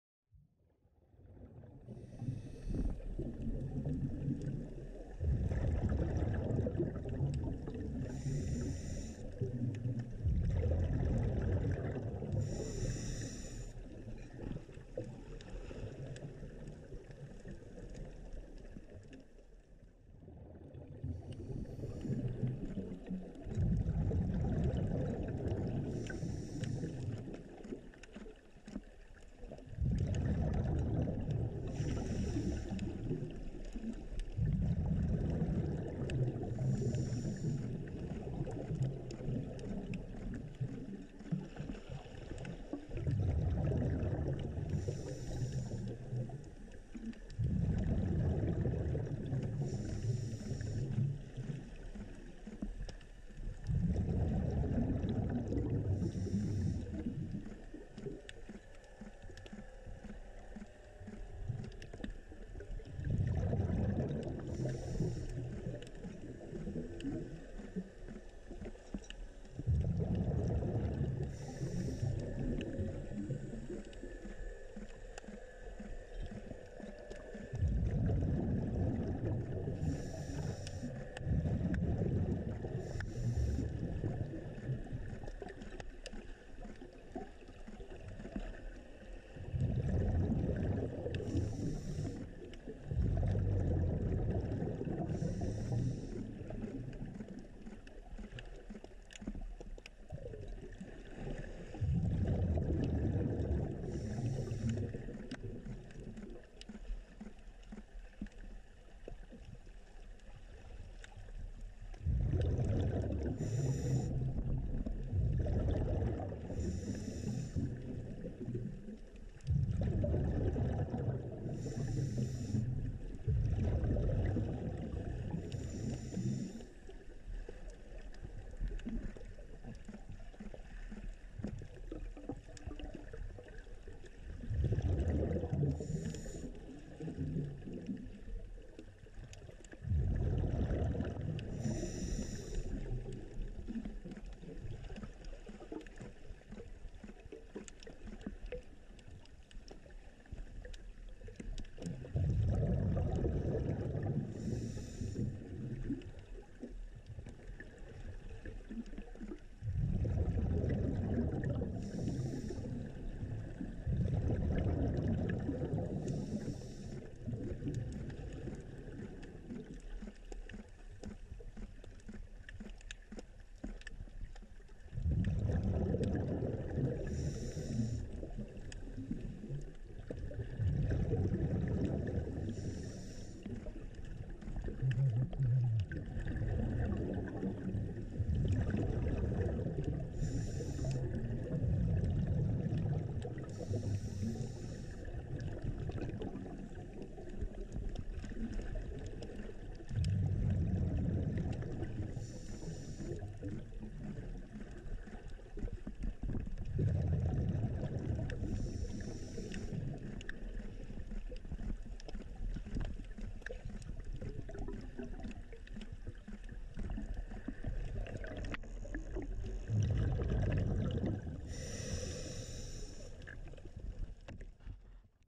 Australian Seal Lions Unedited
These are the true masters of agility.  The video does not lie, and they move with ease.